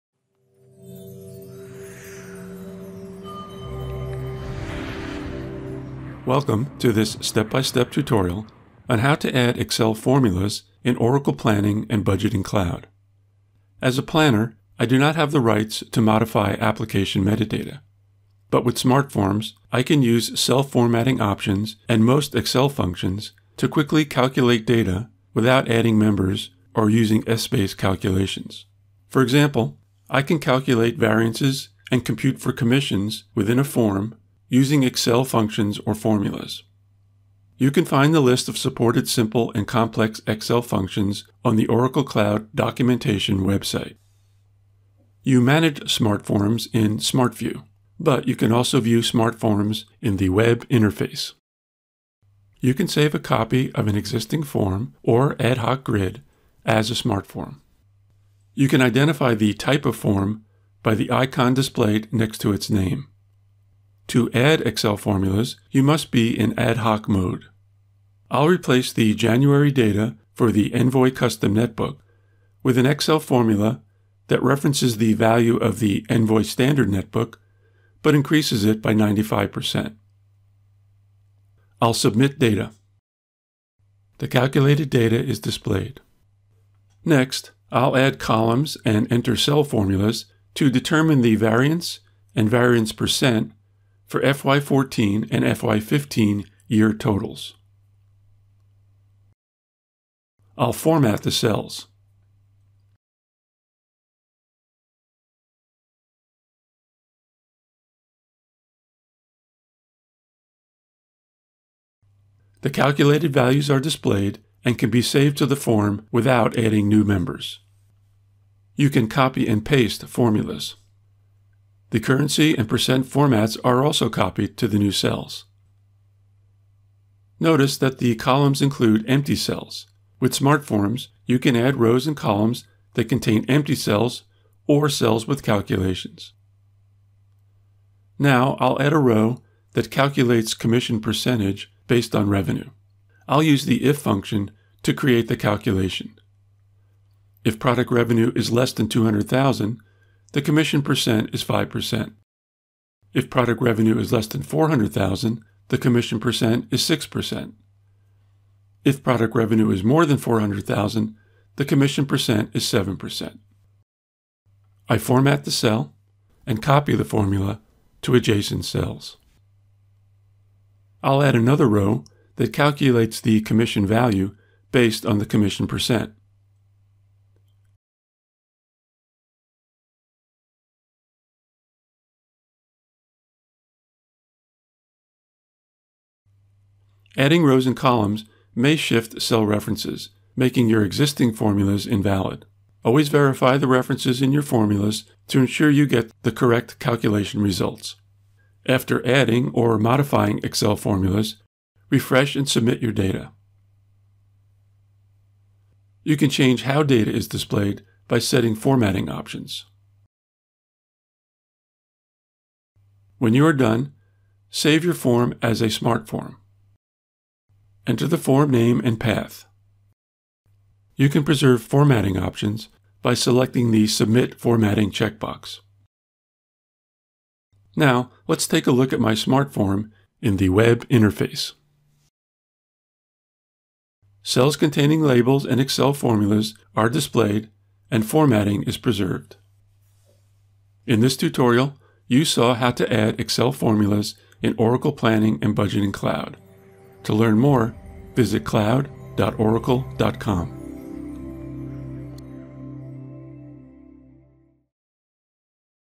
Adding Excel Formulas in Oracle Planning and Budgeting Cloud
This tutorial demonstrates how to write business calculations using Excel formulas in Oracle Planning and Budgeting Cloud. You learn how to add Excel formulas and functions that quickly calculate data in Smart Forms, without adding members or using Essbase calculations.

Planning and Budgeting Cloud is now Planning, a business process in Oracle EPM Cloud